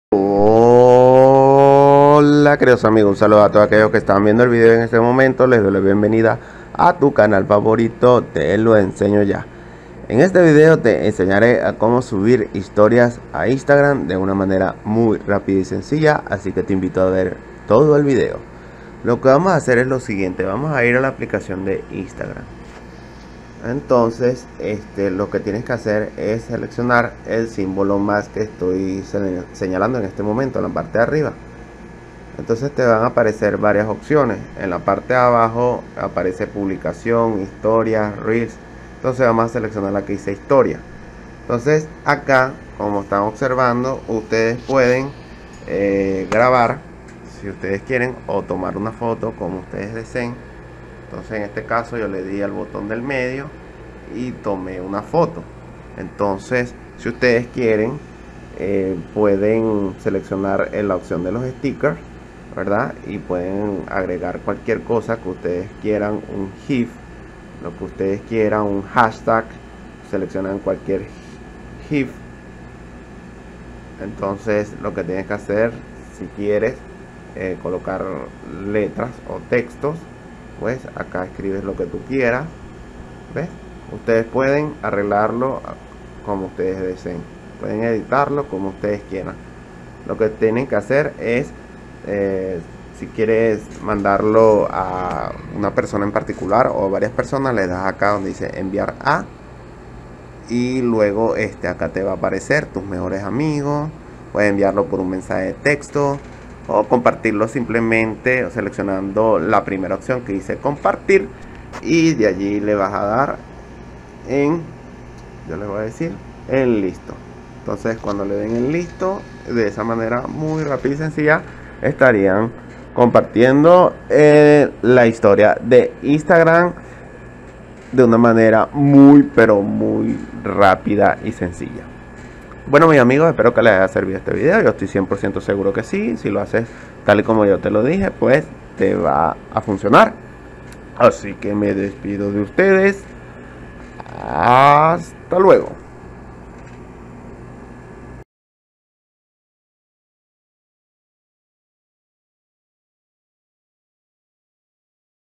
Como Subir Historias de Instagram I Solución 2024
En este video te enseñare a Como Subir Historias de Instagram de una manera rápida y sencilla por medio de varios pasos que cualquier persona puede realizar

Videos Recomendados
💛 Como ACELERAR Celular. Haz esto!